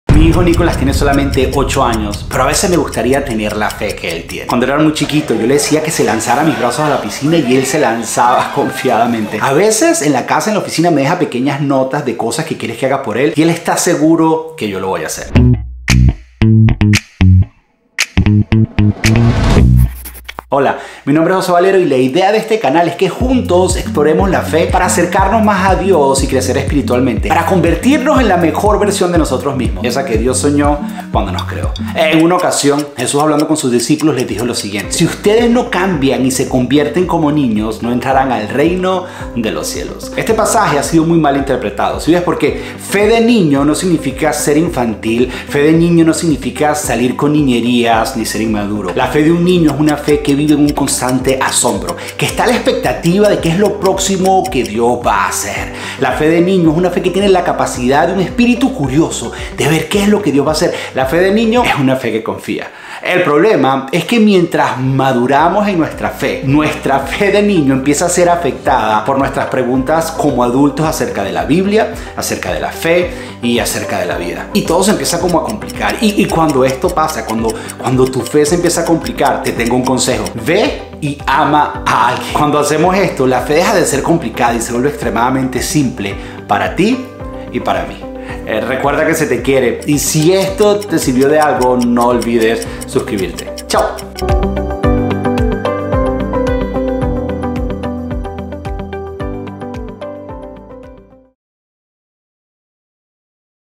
🔴 2 MINUTOS DE FE 🔴 COMO tener LA FE DE UN NIÑO
JESÚS es una ocasión le dijo a sus SEGUIDORES que deberían TENER LA FE de un niño. Pero ¿Qué significa es?

Adquiere mi más reciente LIBRO
"Huellas en en Desierto"